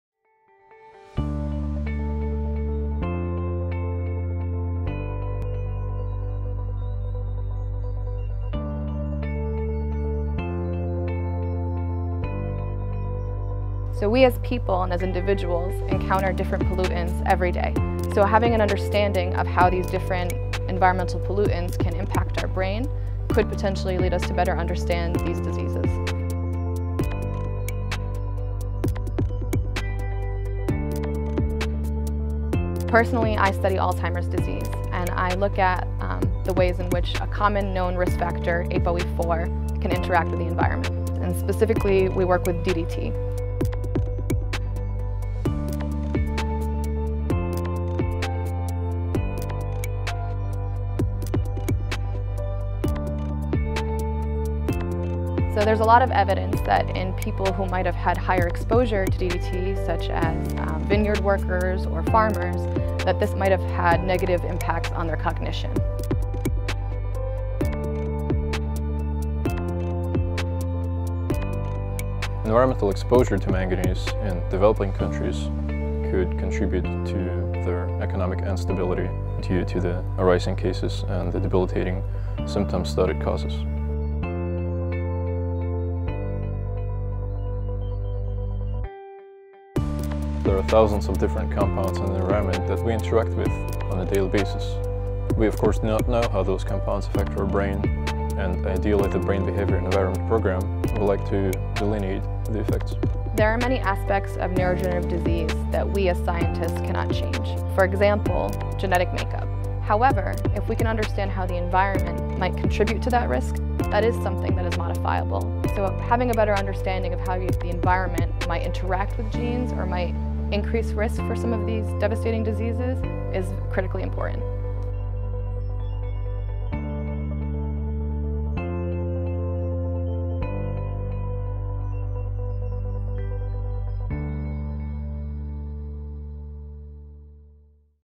FIU's Emerging Preeminent Program: Brain, Behavior and the Environment
The Brain, Behavior and the Environment Program is a transdisciplinary initiative that unites the dynamic and diverse neuroscience community at FIU in the pursuit of three goals: to create and empower research programs focused on environmental causes of neurological disease; to devise strategies and develop treatments for neurological disorders using novel neuroscience and engineering tools as well as pharmacological approaches; and to establish a rich educational resource in South Florida to educate students, faculty, clinicians, the public, and health officials on the role that environmental factors play on neurological disease.